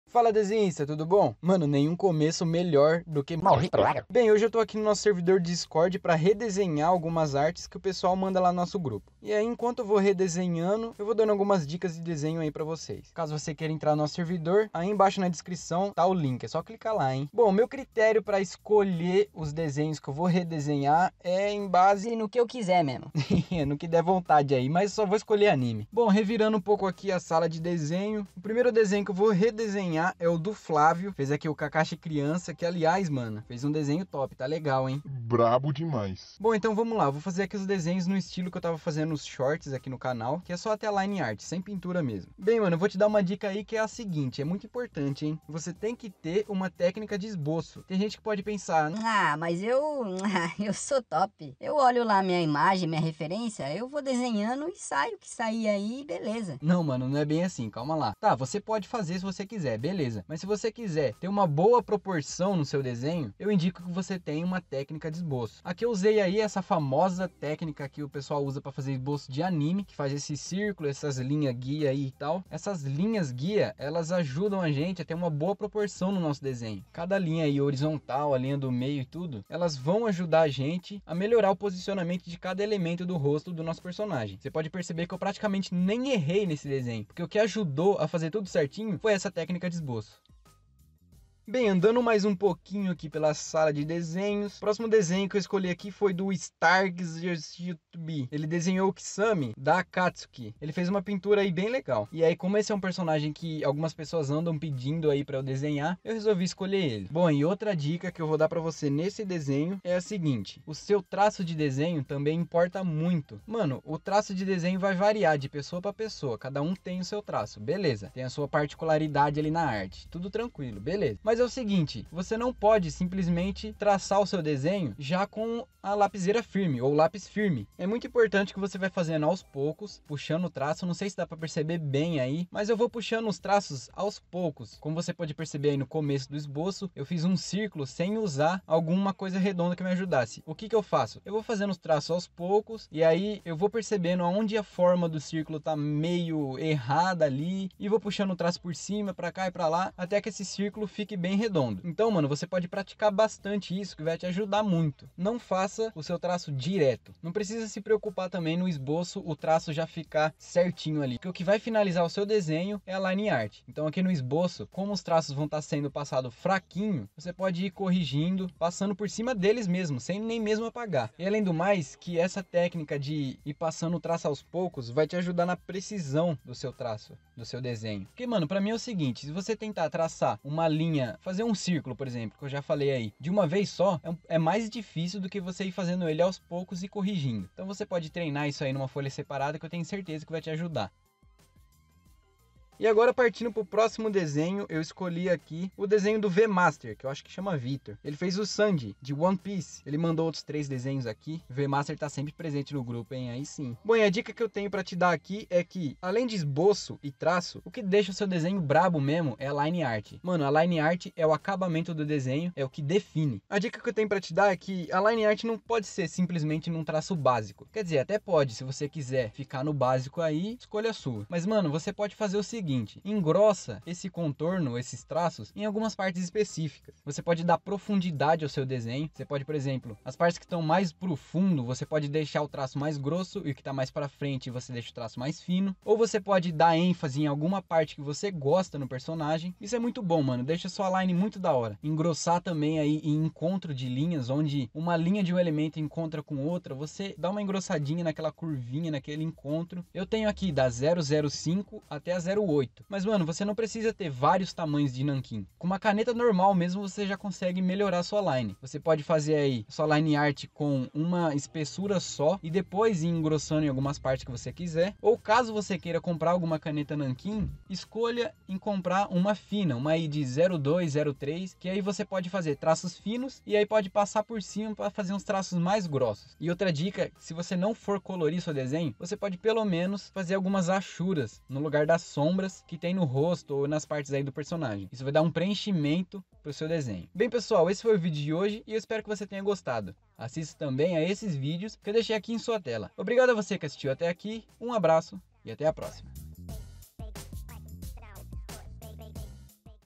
SEU DESENHO BRABO em 3 passos simples | Redesenha
No vídeo de hoje, enquanto redesenho alguns desenhos dos inscritos, dei 3 dicas importantes pra você que quer melhorar o seu desenho!

Basicamente, as dicas que dei hoje vão te ajudar a desenhar melhor se você as treinar bem. Quando falo sobre técnica de esboço, você não precisa necessariamente usar a que mostrei, existem várias outras técnicas pra fazer esboço de anime, sendo elas: Grade, que pode dar um trabalho mas deixa seu desenho ótimo na proporção de cada elemento; Linhas guia, que te dão noção de espaçamento; e Círculo com Linhas Guia, que foi a técnica que usei, a mais popular entre os desenhistas de anime. Você pode usar técnica de decalque também, mas dessa forma você basicamente tá copiando por cima, então não vale muito a pena.

Quanto ao traço de desenho é importante você treinar bastante, isso pode te ajudar de diversas formas, inclusive na precisão em seu desenho. Treinar o traçado com a mão leve e pouca força, te garante um esboço mais trabalhado e que pode dar menos problemas na hora de apagar, quando finalizar o desenho.

A Line Art é quem vai definir seu desenho, então é um momento pra caprichar ao máximo! Tendo um esboço bem feito com as técnicas e um bom traço, a Line vai fechar com chave de ouro, por isso seu contorno não deve ser simples, mas você pode ousar em engrossar e afinar em algumas partes, pra ter um ganho na profundidade e enriquecer detalhes.

Segue as dicas no vídeo pra saber como desenhar melhor!

[Materiais Indicados]:
Nanquim ponta fina Faber Castell

Nanquim 5 pontas Uni-Ball

[Personagens]:
Kakashi Criança - Naruto
Kisame - Naruto
Sanji - One Piece

[Material usado]:
Marcadores BRW Evoke - 24 cores
Caneta Nanquim
Caneta Posca Branca
Folha A5 gramatura 180g

Abraços, até a próxima!